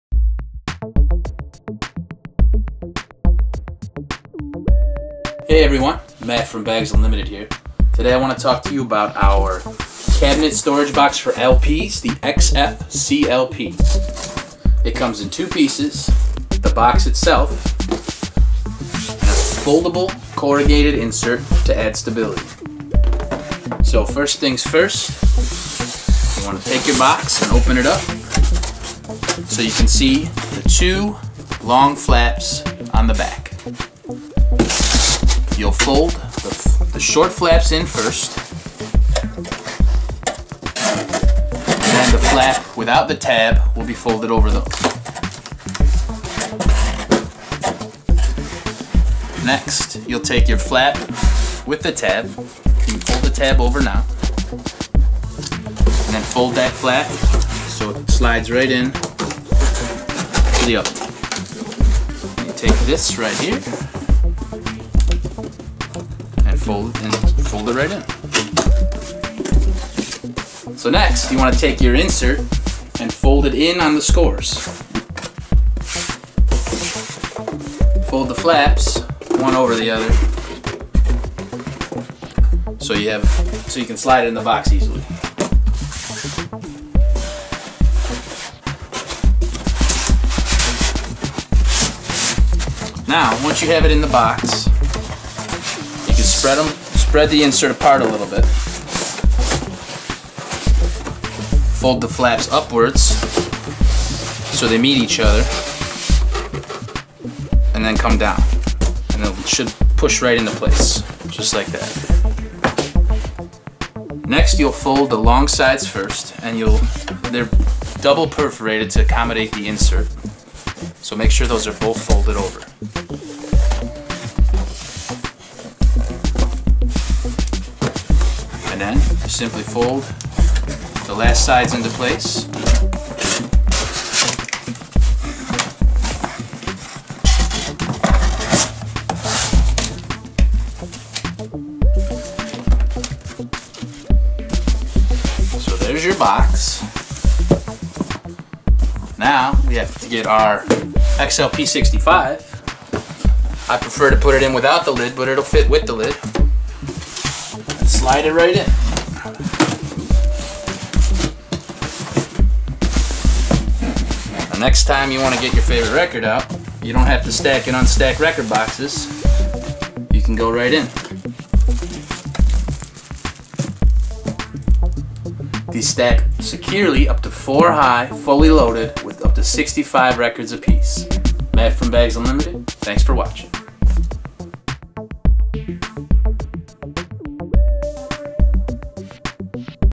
BU's How to Series: Assembling an LP Filing Cabinet
Join Matt as he guides you thru the steps of assembling your very own BU LP Filing Cabinet!

This easy to assemble filing cabinet is available to order

Love collecting records? So do we! Join our Record Collector Community on Facebook today!